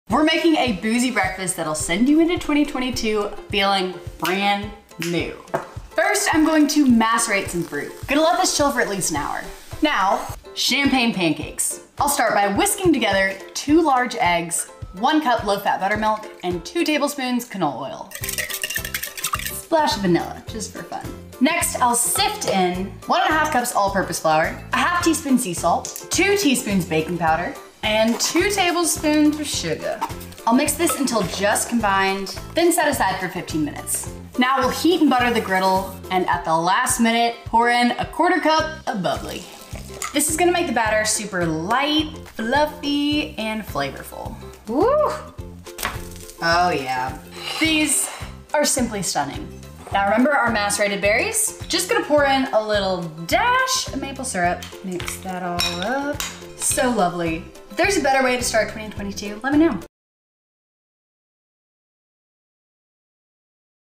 How To Make Champagne Pancakes
What do you do with that leftover Champagne after New Year's Eve or brunch? Make pancakes of course! These taste delicious with macerated fruits and accompanied by a glass of champagne.